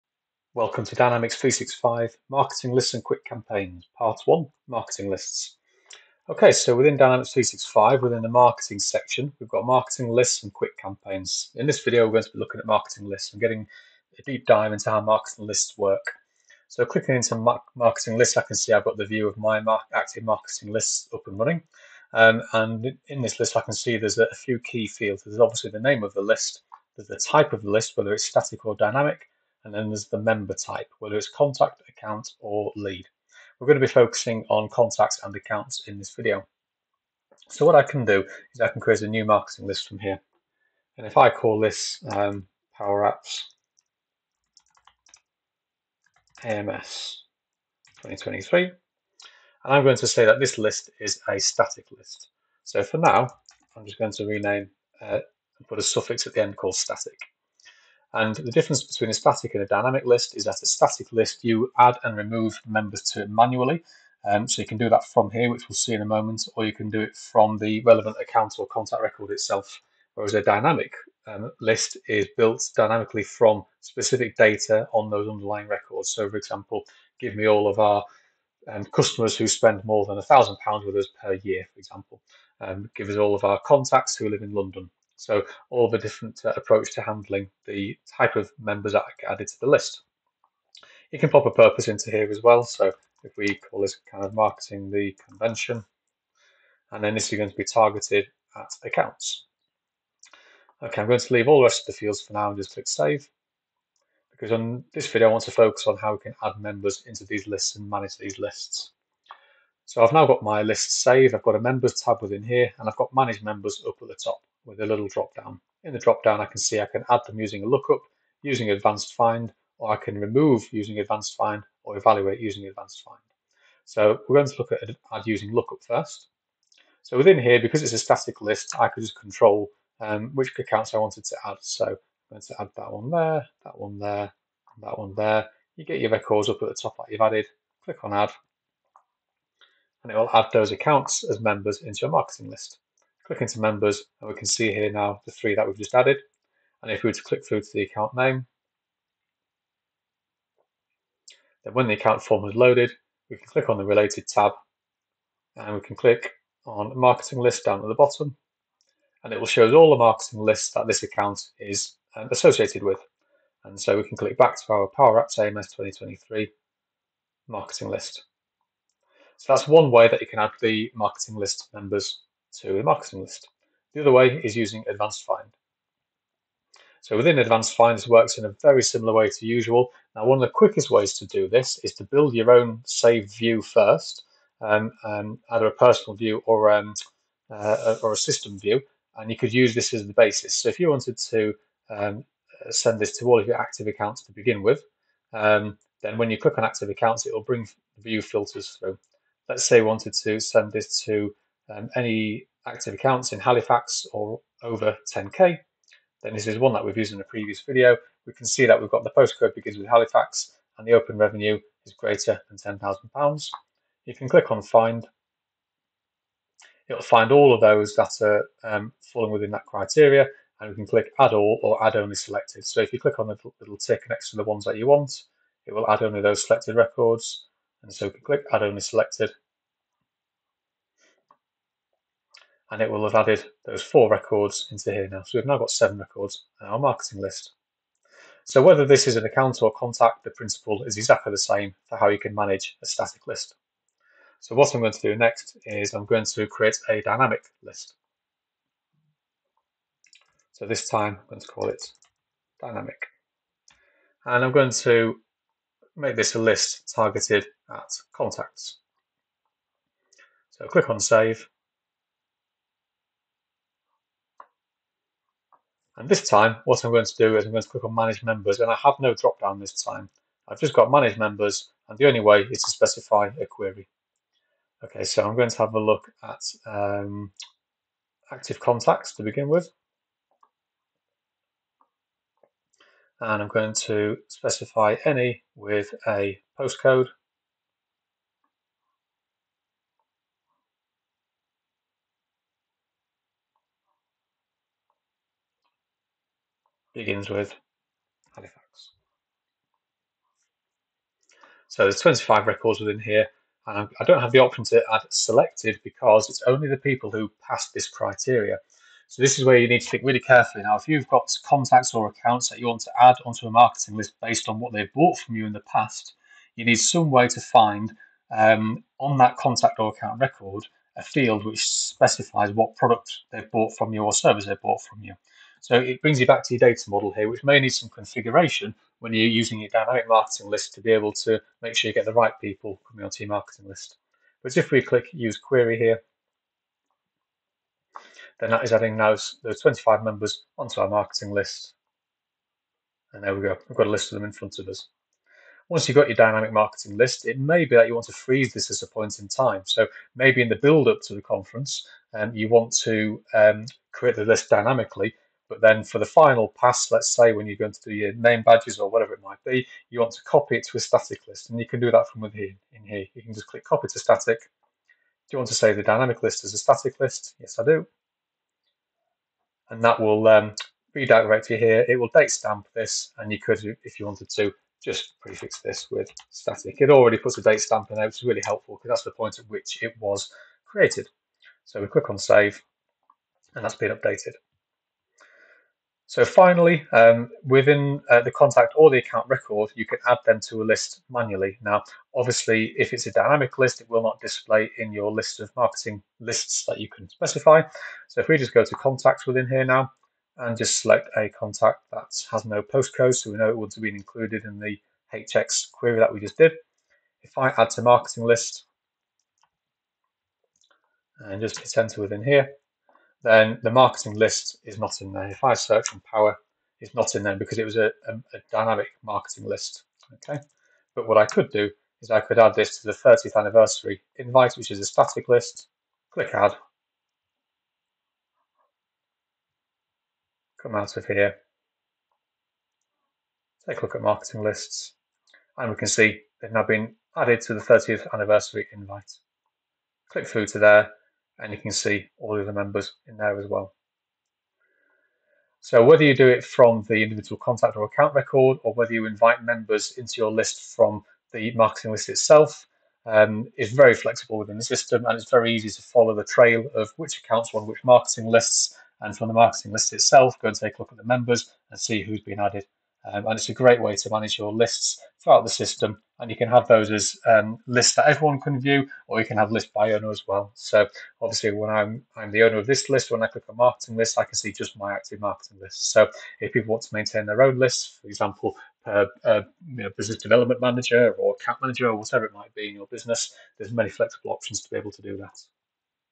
How to Use Marketing Lists: Beginners Dynamics Sales
How to create emails (and other activities) in bulk to send out from Dynamics and create a history in the Timeline.

In this 8 minute video we’re diving into Marketing Lists.